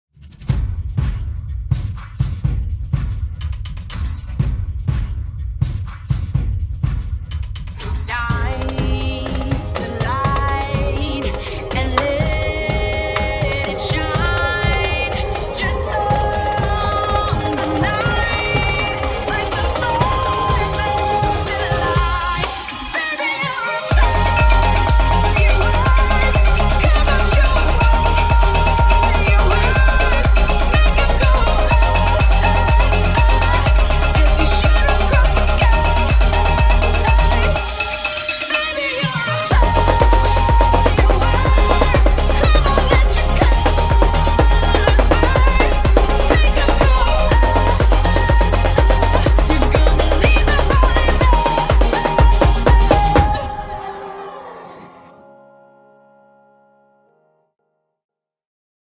Modern LED Drummers - AliveNetwork.com
Delivering exceptional drumming skills, modern LED technology and a unique, high impact show, Modern LED Drummers have performed for some of the world's biggest names! Creating an unrivalled party atmosphere, your guests will be up on their feet watching in awe!